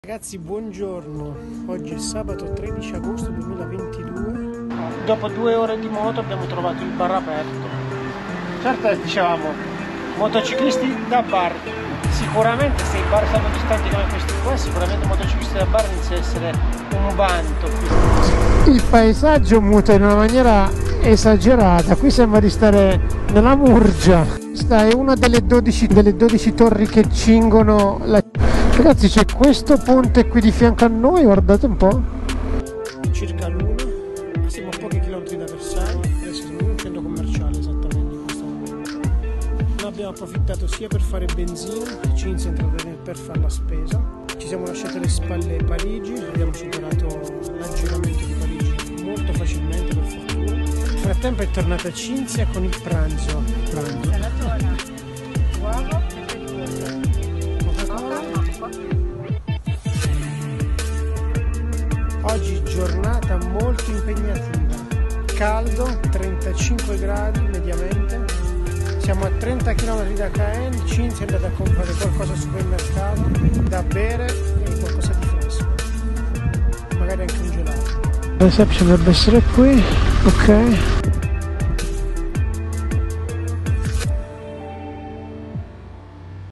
💊 IRLANDA ☘️ 2022 🇮🇪 ✅ DAY 2️⃣ - 13 AGOSTO 📌
Buongiorno ieri altra giornata colma di km: 570 circa 🚀
Zero km di autostrada 😁
Temperatura media 34.5 gradi con punte di 36 🥵
Domani ore 15.00 si salpa 🛳 x l’Irlanda ☘️
Buona serata 🤙🏼